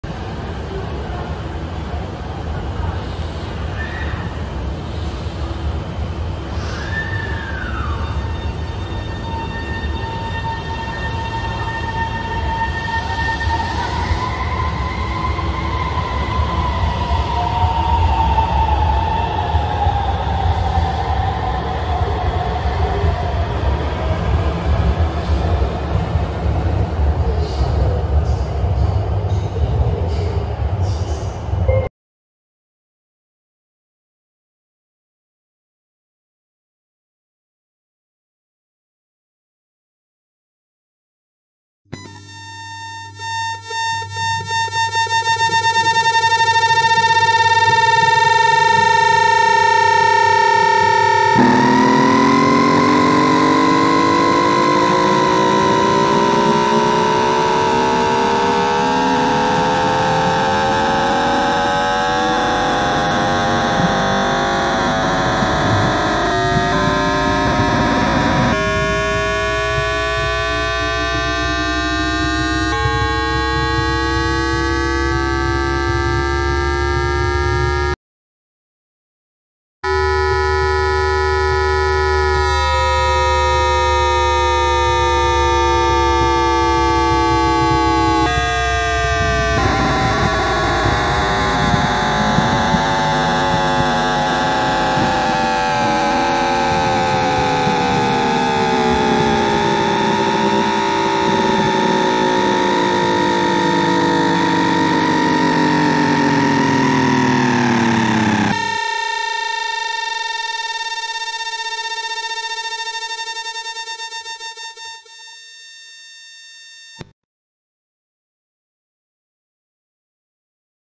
[Times Electric VVVF] Inductor-Recorded VVVF sound Localized Train on Beijing Changping Line
Because I simultaneously posted it on Bilibili, there are both Chinese and English in the video.

A similar modulation was tested on train 02001 (Line 2, but was changed later right before carrying passengers.